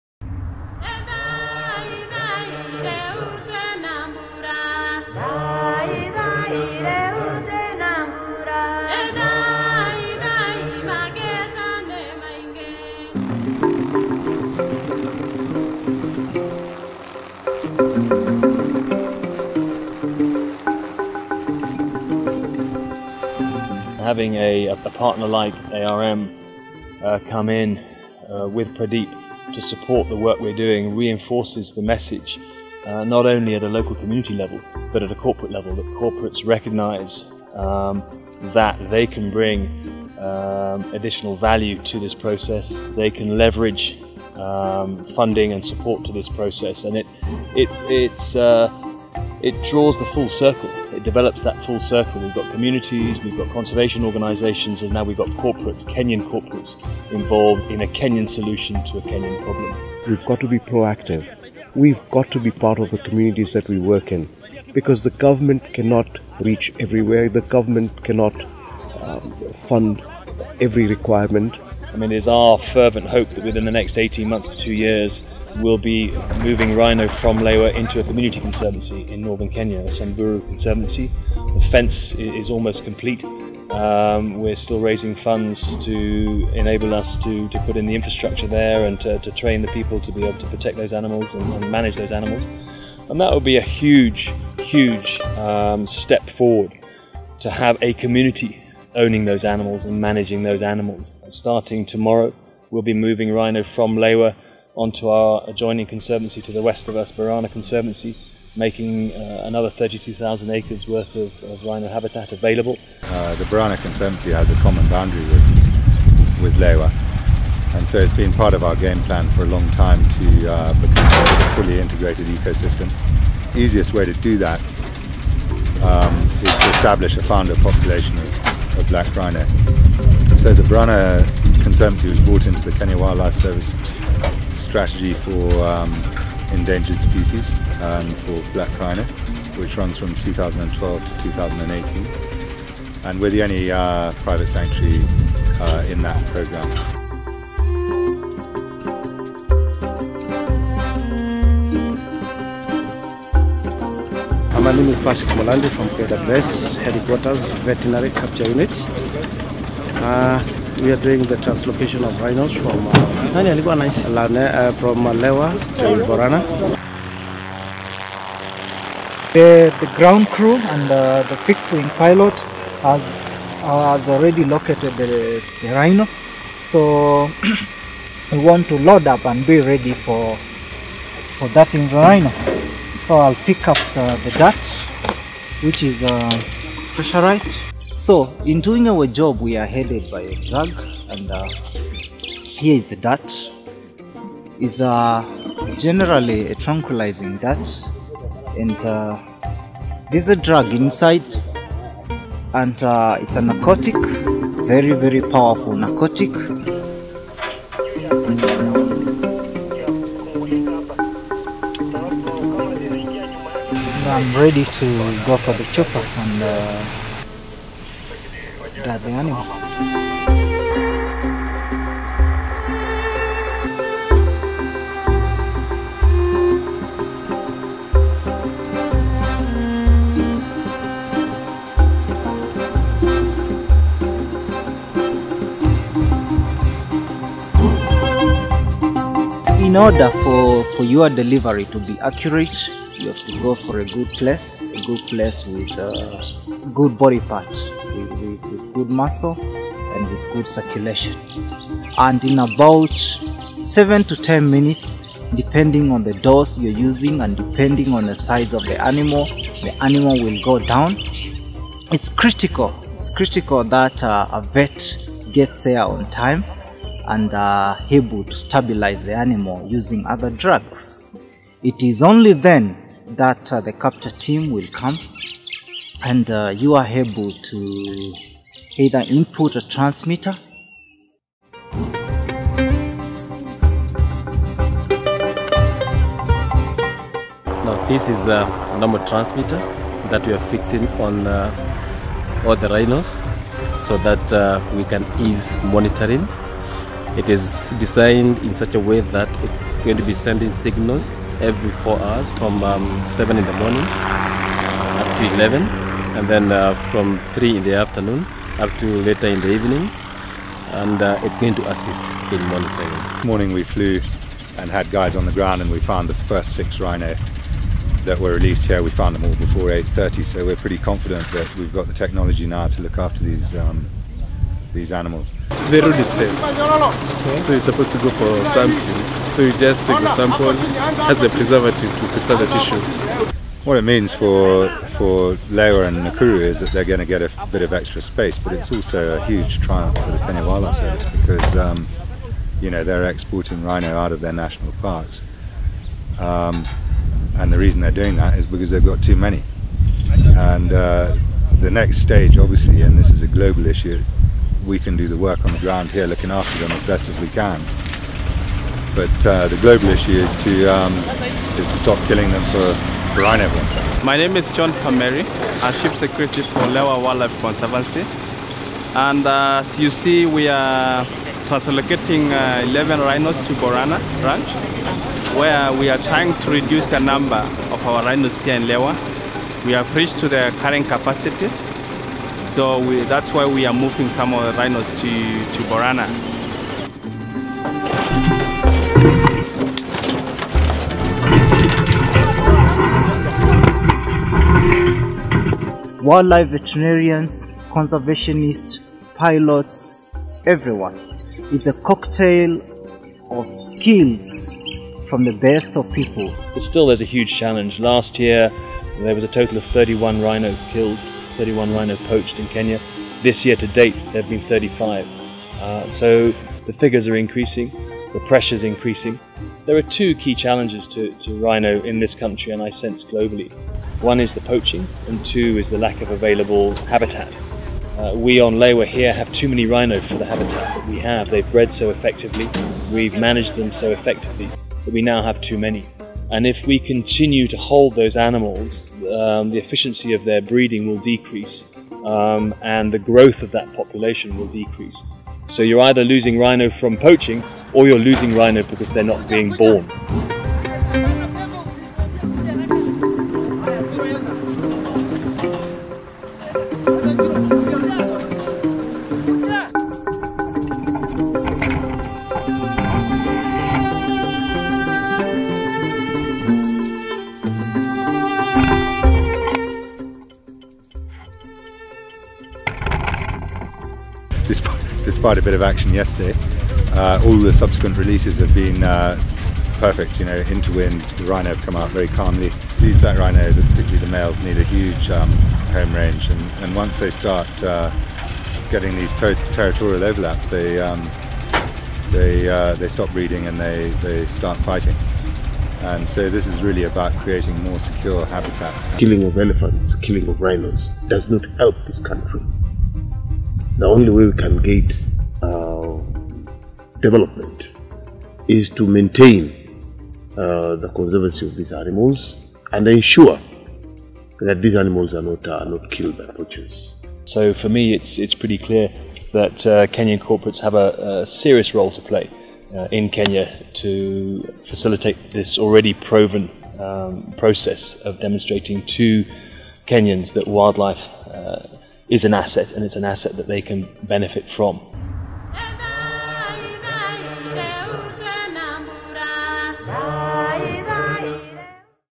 Moving Rhino to Establish Sanctuary at Borana Conservancy
In 2013, Lewa, its western neighbour Borana Conservancy and the Kenya Wildlife Service collaborated to expand black rhino habitat in Kenya by establishing a new sanctuary at Borana.

Last year, the two conservancies took another unprecedented step and removed the dividing fence to form one of the biggest rhino landscapes in East Africa, 93,000 acres of prime black rhino habitat.

Support by a Kenyan corporate organisation, Rhino Cement Company, and local government reinforces the message that Kenyans are indeed willing to invest in the management of their country's most endangered species.

Rhino translocation filmed on location at Lewa and Borana Conservancy by Mark Muinde and Teeku Patel of Sokomoto Images.